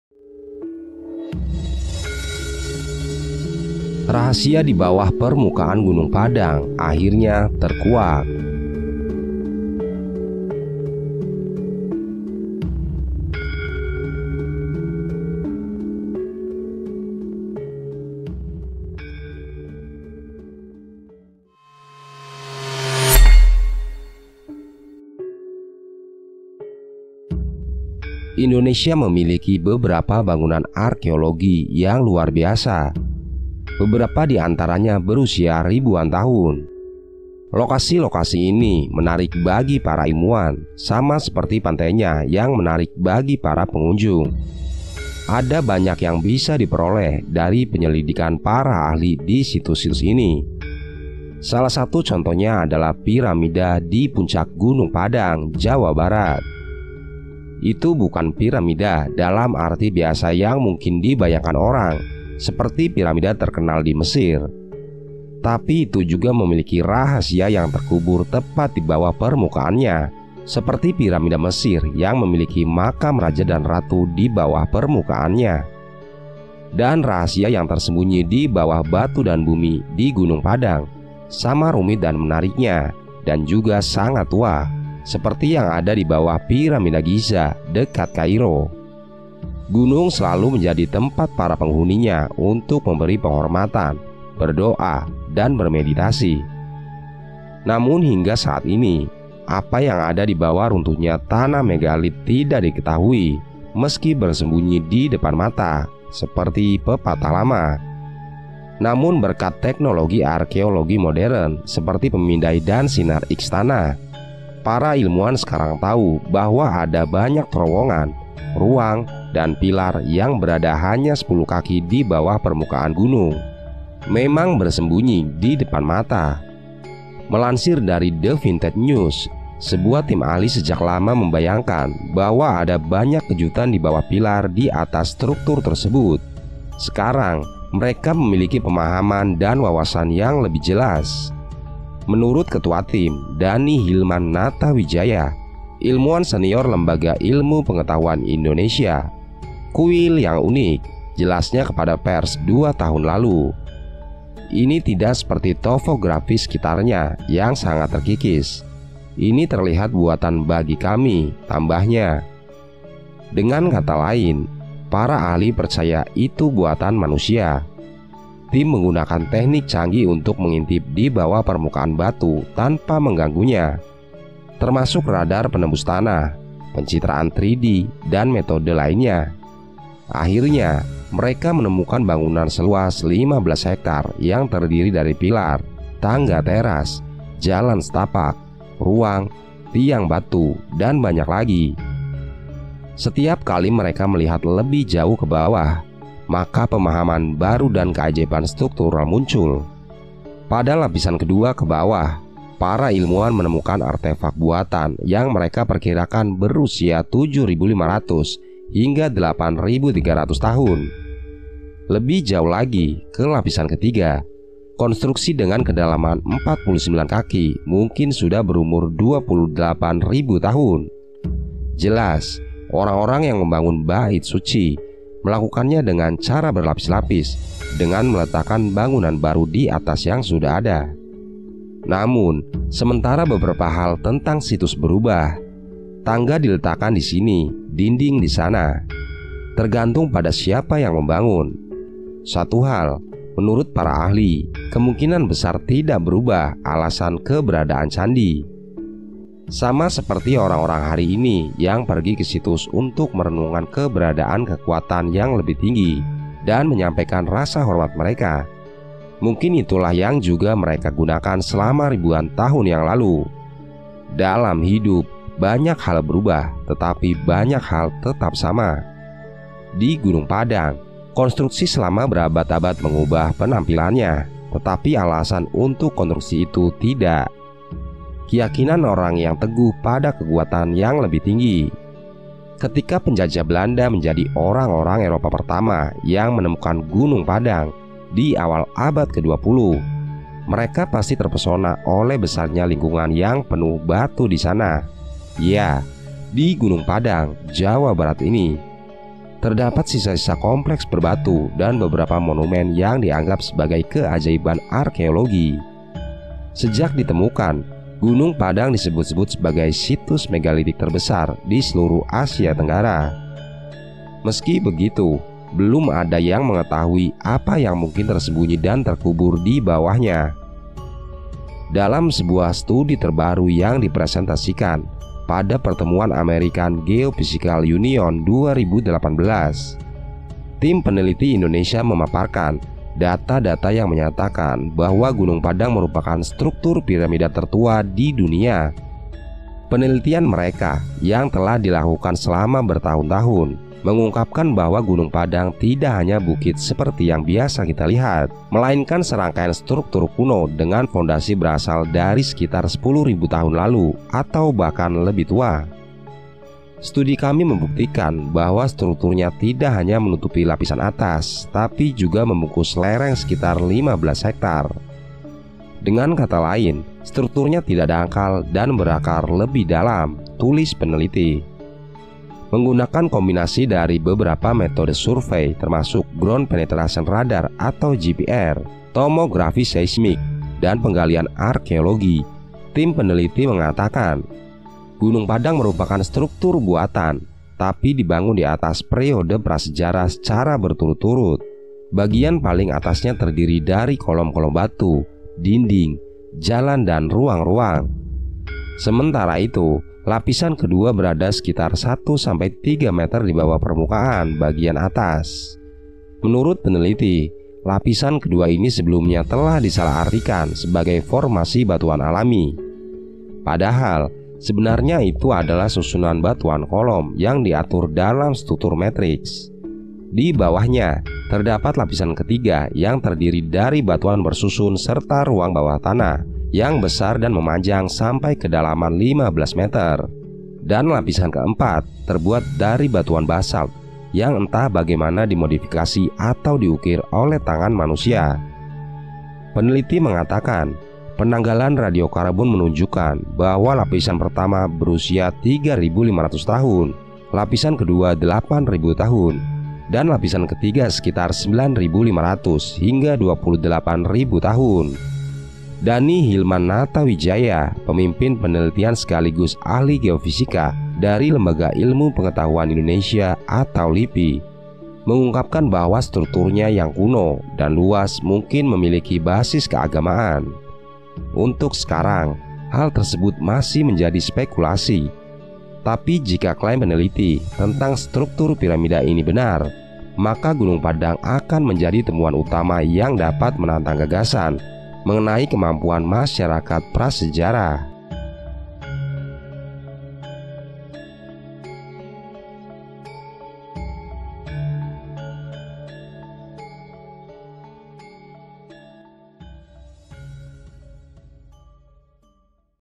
BIKIN BANGGA ❗❗ INDONESIA BIKIN SEMUA ILMUAN DUNIA KAGET❗ SITUS GUNUNG PADANG GUNCANG DUNIA
Rahasia di Bawah Permukaan Gunung Padang Terkuak,

Indonesia memiliki beberapa bangunan arkeologi yang luar biasa, beberapa di antaranya berusia ribuan tahun.
Lokasi-lokasi ini menarik bagi para ilmuwan sama seperti pantainya menarik bagi pengunjung.
Ada banyak yang bisa diperoleh dari penyelidikan para ahli di situs-situs ini.
Salah satu contohnya adalah “piramida” di puncak Gunung Padang, Jawa Barat.
Itu bukan piramida dalam arti biasa yang mungkin dibayangkan orang, seperti piramida terkenal di Mesir.
Tapi itu juga memiliki rahasia yang terkubur tepat di bawah permukaannya, seperti piramida Mesir yang memiliki makam raja dan ratu di bawah permukaannya.
Dan rahasia yang bersembunyi di bawah batu dan bumi di Gunung Padang sama rumit dan menariknya - dan juga sangat tua - seperti yang ada di bawah Piramida Giza dekat Kairo.
Gunung selalu menjadi tempat para penghuninya untuk memberi penghormatan, berdoa, dan bermeditasi.
Namun hingga saat ini, pada tahun 2018, apa yang ada di bawah runtuhnya tanah megalit tidak diketahui, meski “bersembunyi di depan mata”, seperti pepatah lama.
Berkat teknologi arkeologi modern, seperti pemindai dan sinar-X tanah, para ilmuwan sekarang tahu bahwa ada banyak terowongan, ruang, dan pilar yang berada hanya 10 kaki di bawah permukaan gunung.